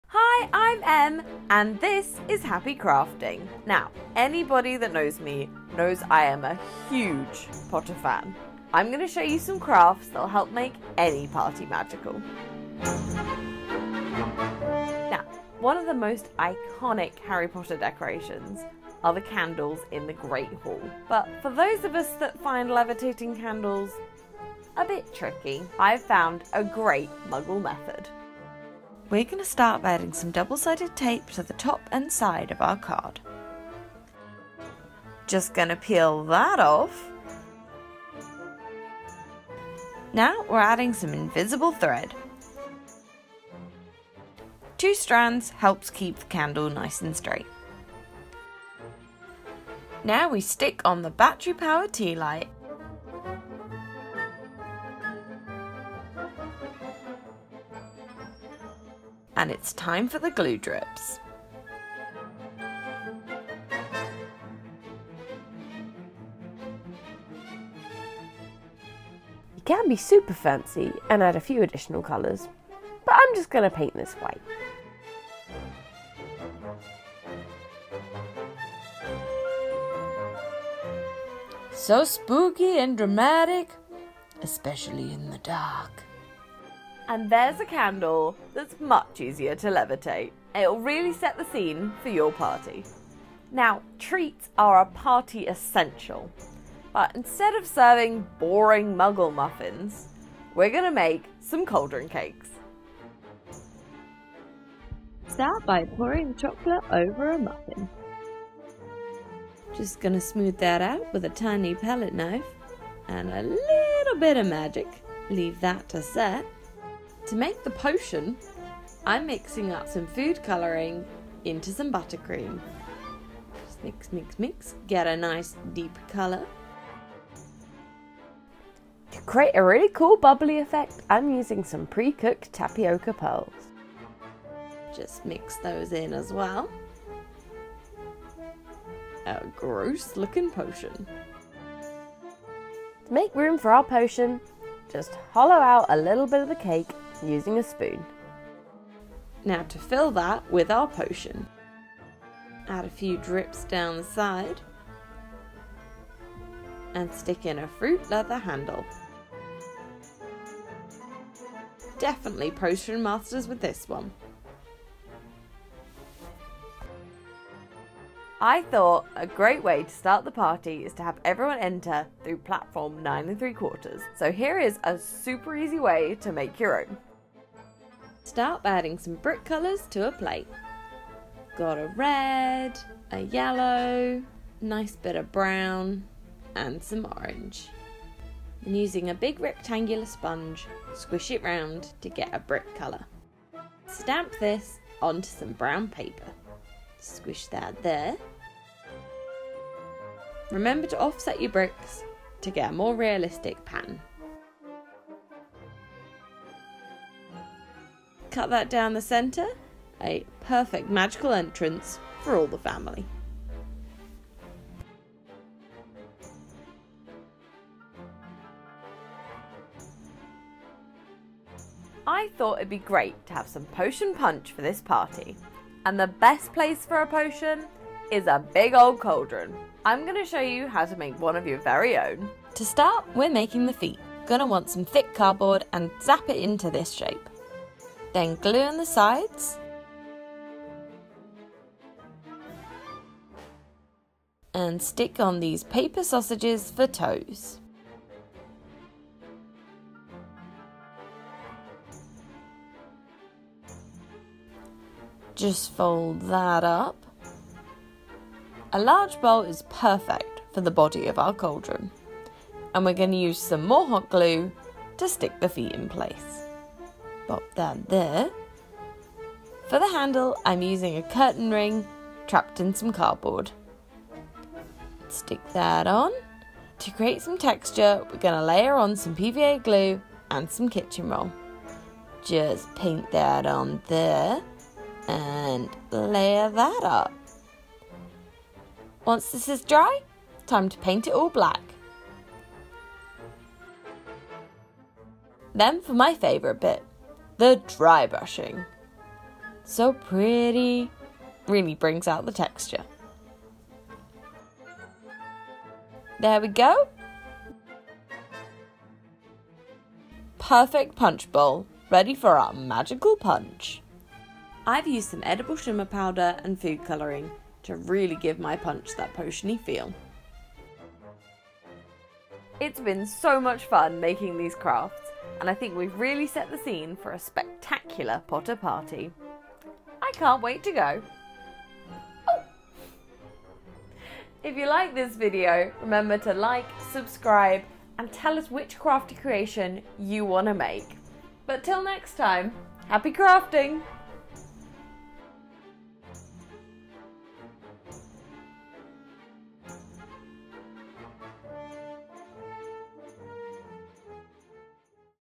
4 Incredible Harry Potter Crafts | Happy Crafting
We love Harry Potter, and we know you do too! Today, Em will be showing you how to bring Hogwarts into your home with these 4 incredible Harry Potter crafts. Happy Crafting everyone!